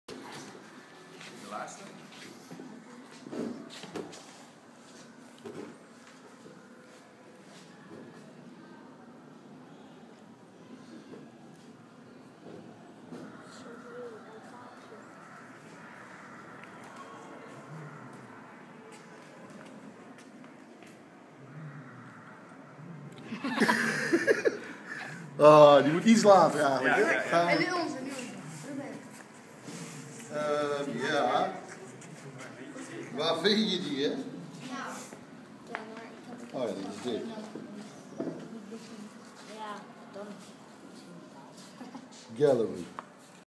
Ruben van der Meer is STOER met kids op Cinekid workshop fi
Ruben van der Meer is STOER met kids op Cinekid workshop film acting